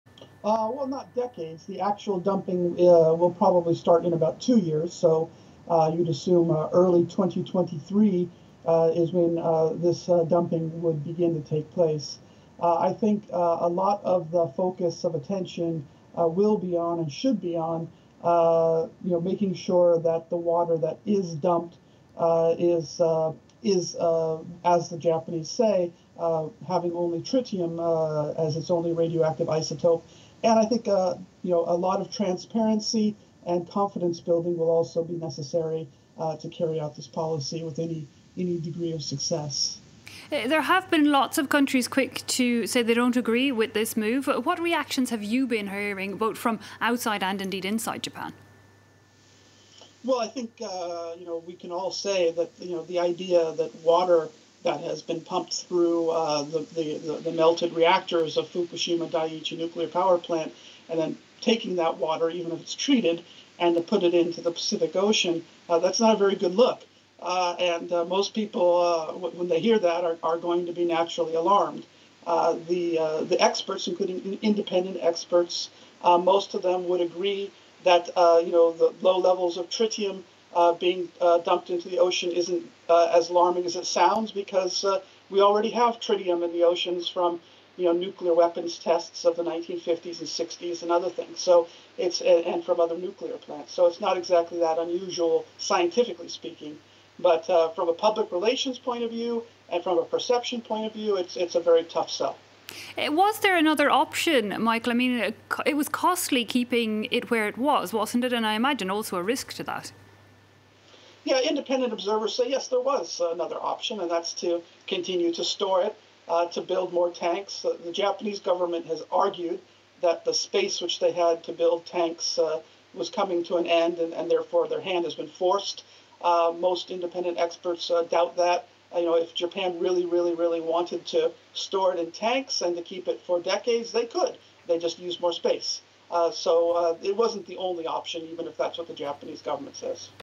Japan to dump contaminated water from Fukushima nuclear station into sea
Japan plans to release into the sea more than 1 million tonnes of contaminated water from the destroyed Fukushima nuclear station, the government said on Tuesday.